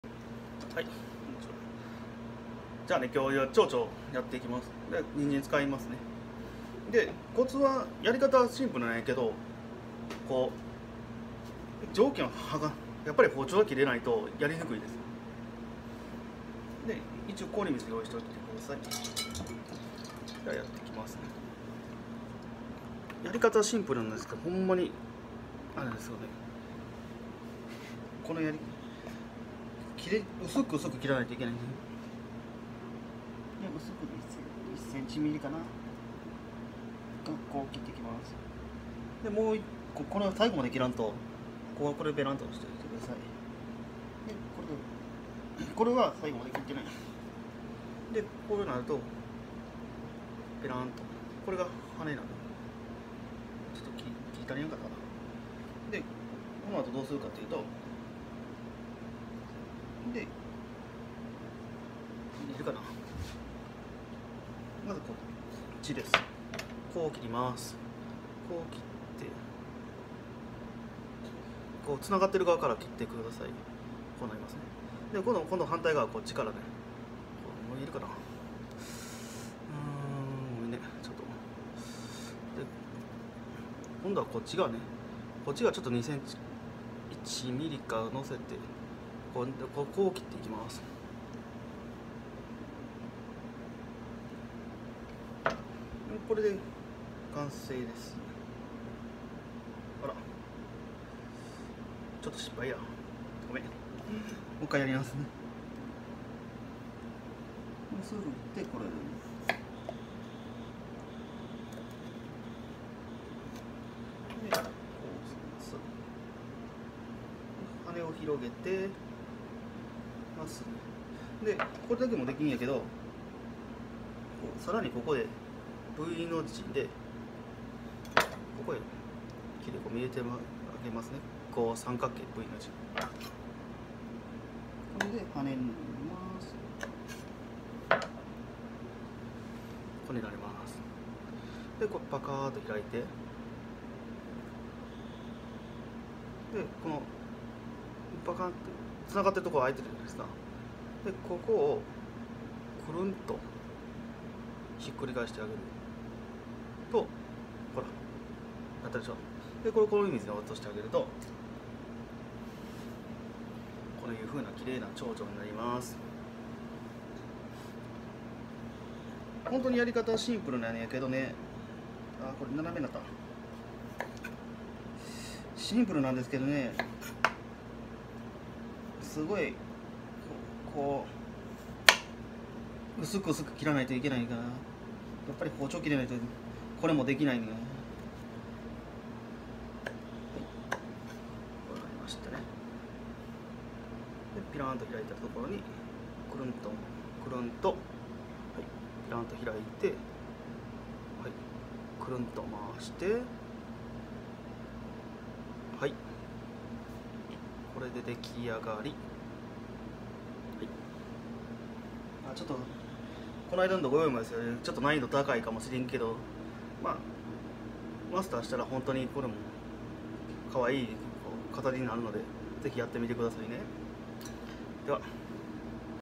人参で蝶々の飾り切り
人参で🥕蝶々🦋飾り切りを解説しました。一本取りの失敗ありです。💦ハプニング